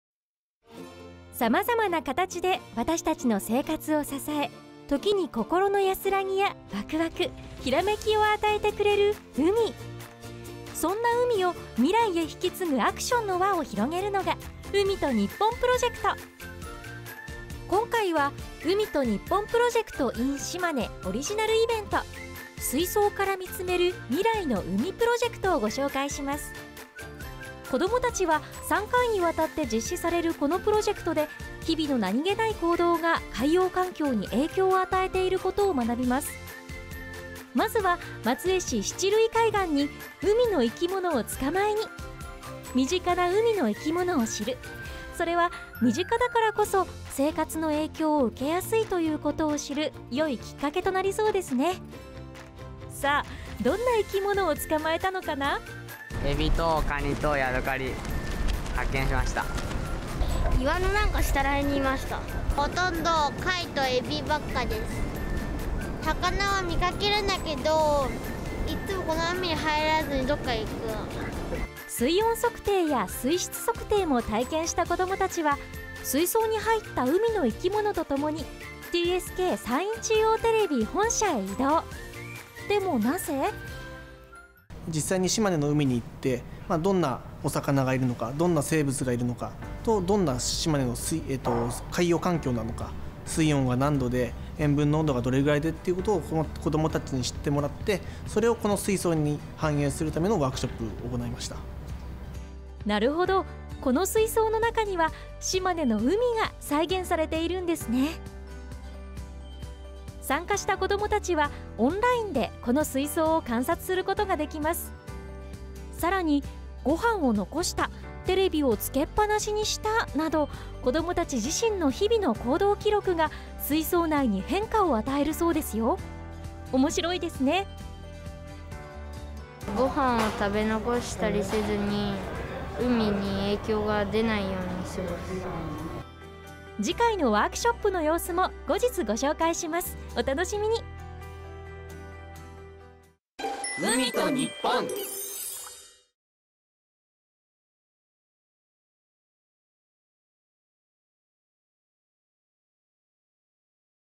水槽から見つめる未来の海プロジェクト 日本財団 海と日本PROJECT in しまね 2020 #30
「水槽から見つめる 未来の海プロジェクト」は3回に渡って実施される小学生対象の学習プログラムです。このプログラムで子ども達は日々の何気ない行動が海洋環境に影響を与えている事を学びます。松江市七類港の海岸で捕まえた生物を水槽へ入れ、TSK山陰中央テレビのロビーへ設置。オンラインで観察をしてもらうのですが、ここに面白い仕掛けが。「ご飯を残した」「テレビをつけっぱなしにした」など、子ども達自身の日々の行動が水槽内に変化を与えます。

・会社名
・部署名
・お名前